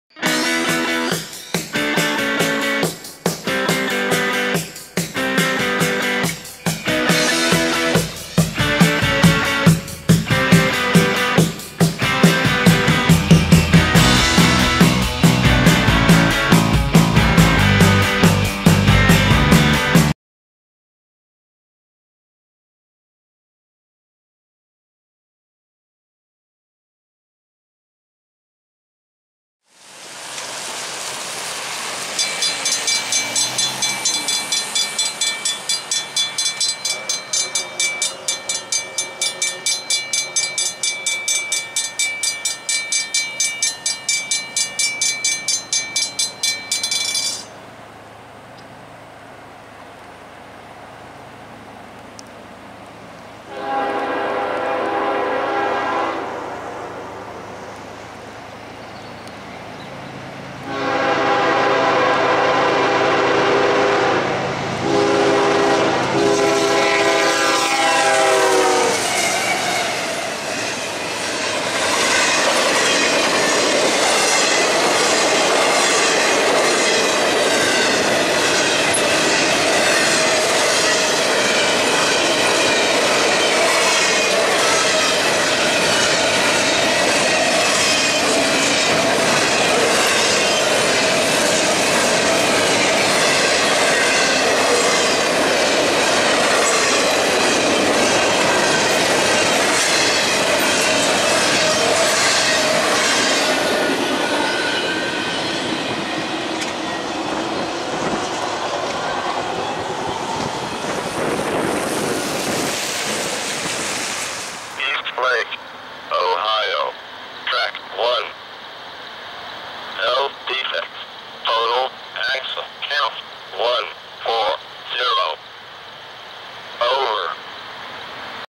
CSX Intermodal Rolling Through Downtown Madison - Madison, OH
This spot sucked. I am never filming here again for the record.